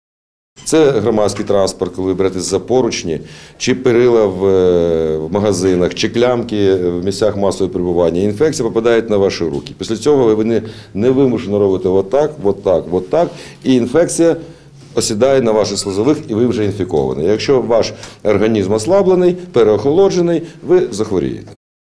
За тиждень майже 4 тисячі львів’ян захворіли на ГРЗ
Якщо у холодну погоду не одягатися тепло або на додачу ще й промочити ноги – хвороби не уникнути. Як стверджують медики, за останній тиждень через похолодання кількість хворих на ГРЗ збільшилася: захворіло близько 4 тисяч львів’ян, майже половина з них – діти віком від 1 до 9 років.

Якщо за два-три дні стан хворого не покращується, обов’язково треба звернутись до лікаря. А от ходити на роботу чи водити дітей до школи і в дитсадок не можна навіть за перших ознак застуди – щоб не наражати на хворобу інших. Проте найголовніша порада для всіх – частіше мити руки, бо саме через них передається інфекція. «У громадському транспорті ви беретесь за поручні чи за перила в магазині, чи за клямки на дверях в місцях масового користування. Інфекція потрапляє на ваші руки, після цього ви підсвідомо робите невимушений рух по обличчю – і так інфекція осідає на ваших слизових, ви вже інфіковані. Якщо ваш організм ослаблений, ви захворієте», – розтлумачив начальник міського управління охорони здоров’я Володимир Зуб.

Крім цього, запевняють медики, ще не пізно вакцинуватись. Зробити це варто найперше тим, хто працює з великою кількістю людей, а також хронічно хворим і літнім людям, яким понад 60 років.